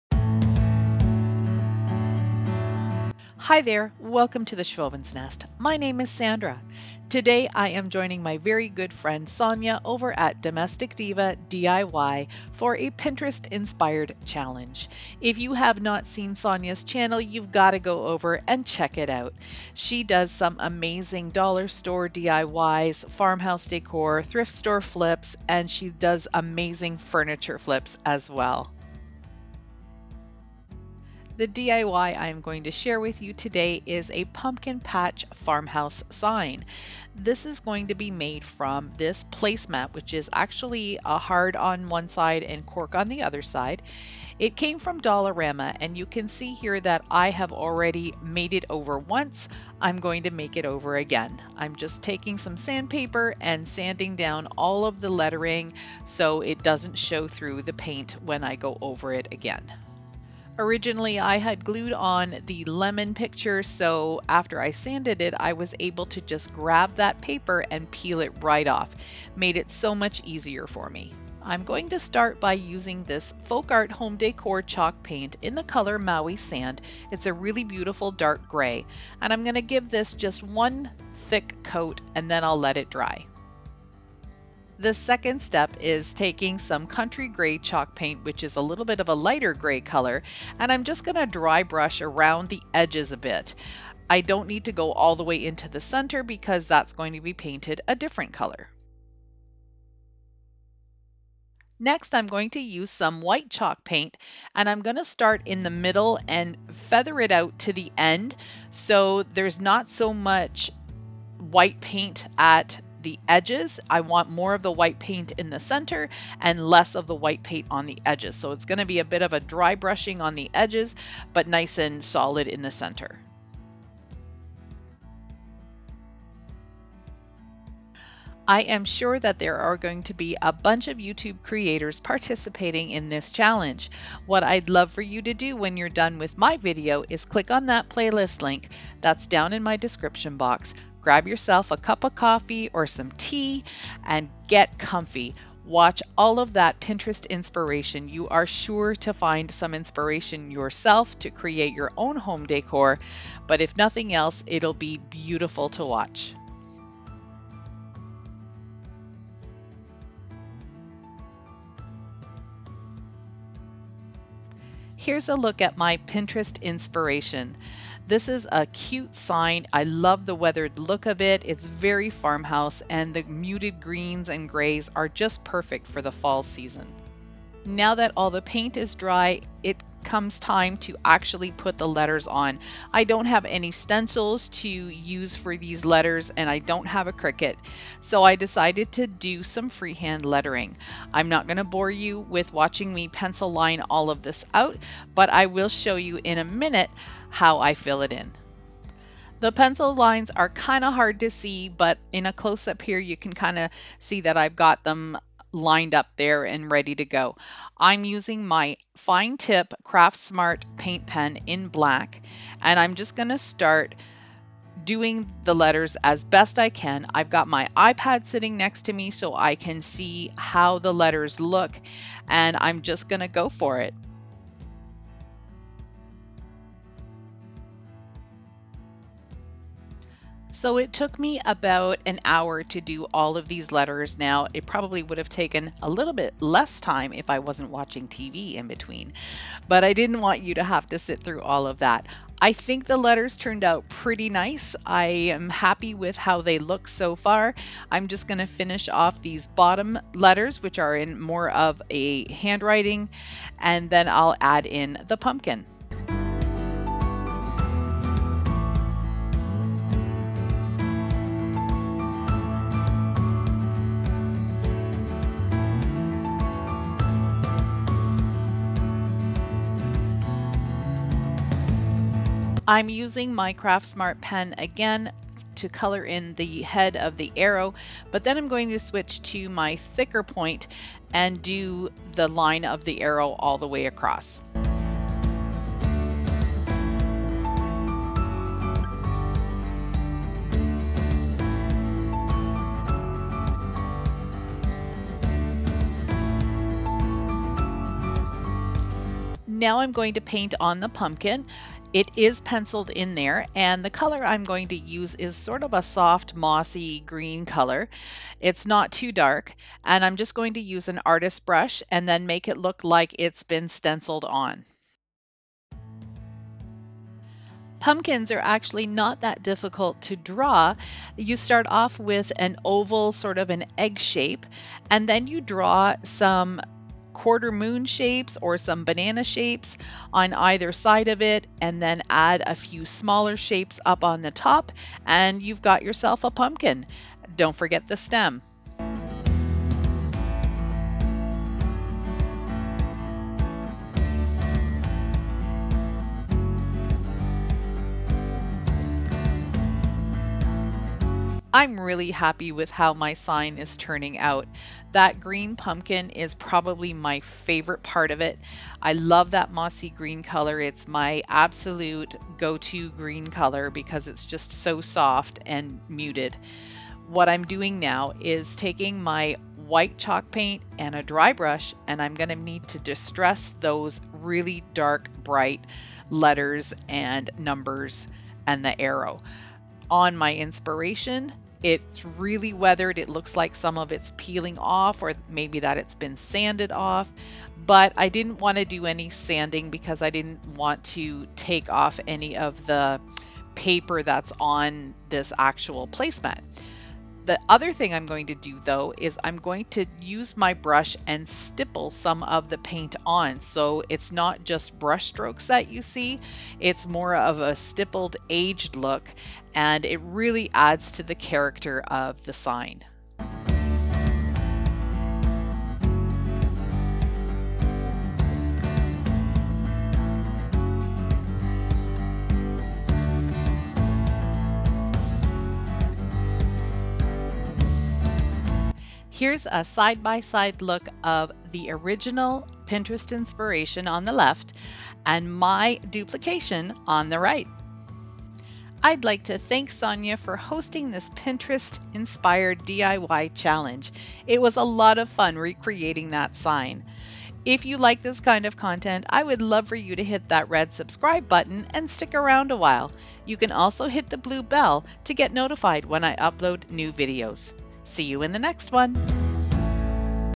Farmhouse Fall Decor~Dollarama Pumpkin Patch Vintage Sign~Pinterest Inspired Challenge
See how I made this sign from a pinterest inspiration photo! This video is part of Domestic Diva DIY's pinterest inspired challenge. Check out her channel and the playlist below! Enjoy!